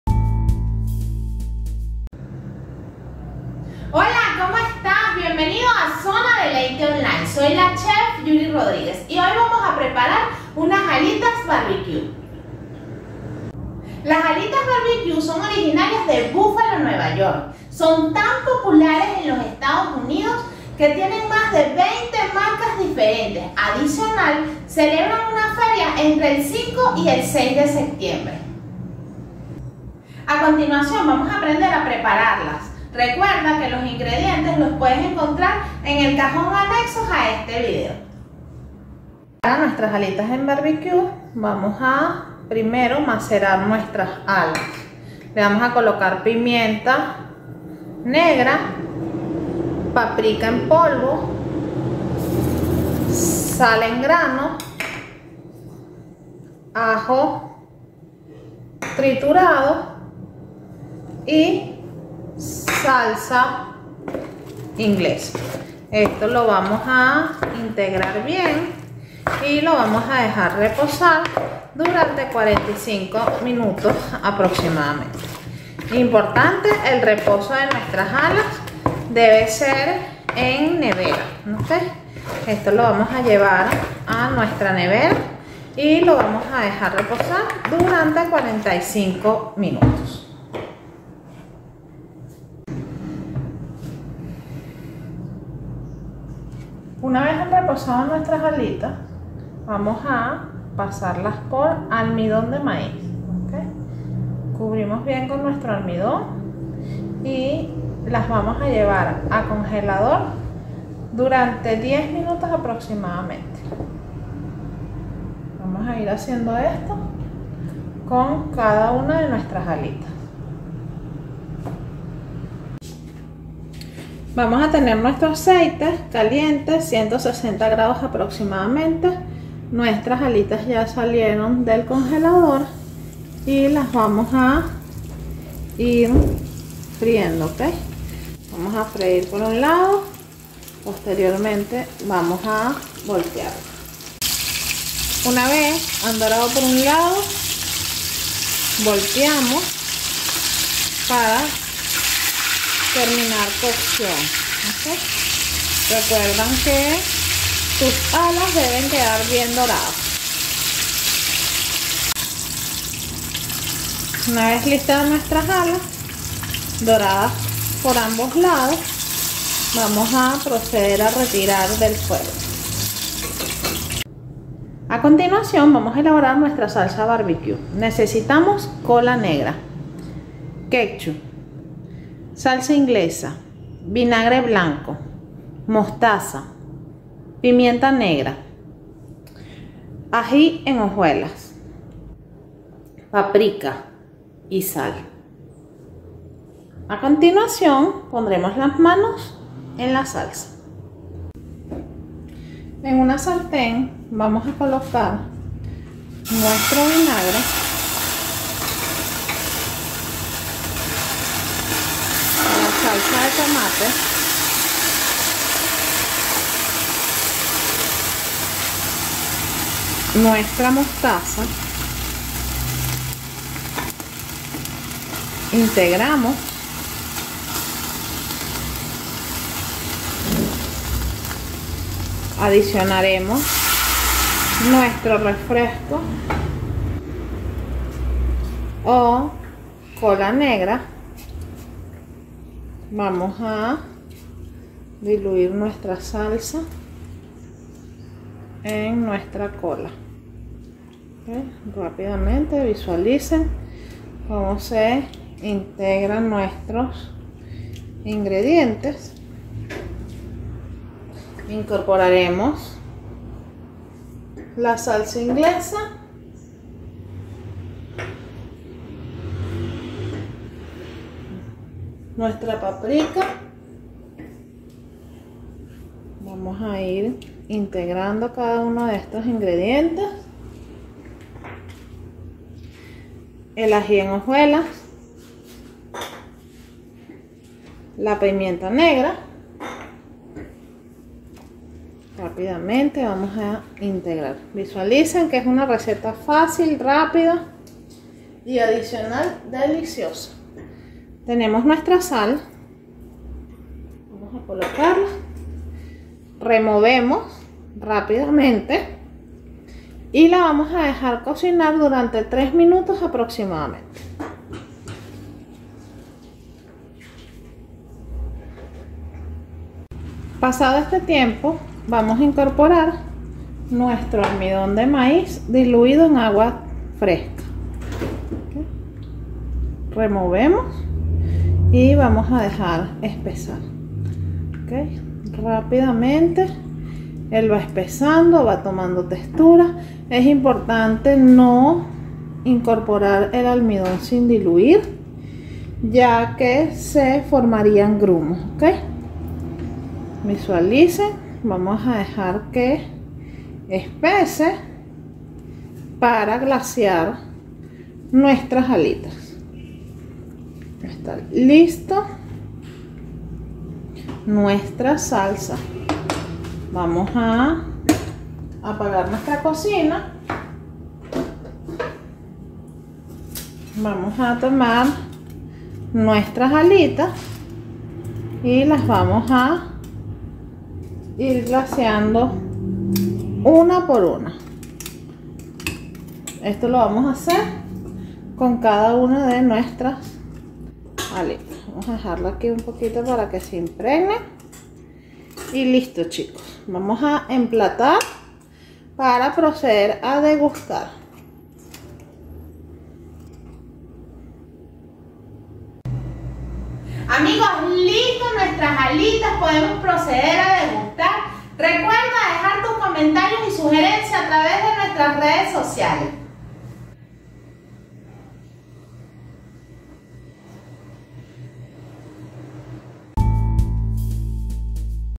Cursos de Cocina Online - Festival Cocina para Principiante - Alitas de pollo BBQ
Ingredientes que debes tener para preparar las Alitas de pollo BBQ:

Para macerar las alitas:
1/2 kilo de alitas de pollo
2g de pimienta negra
2g paprika en polvo
2g sal gruesa
4 dientes de ajo triturado.
45g de almidón de maiz

Para la salsa barbecue:
100g de cola negra
100g de salsa de tomate
5g de mostaza
2g de pimienta negra
5g de vinagre blanco
5g de salsa inglesa
2g de paprika
3g de ají en hojuelas
2g de sal
10g de almidón de maíz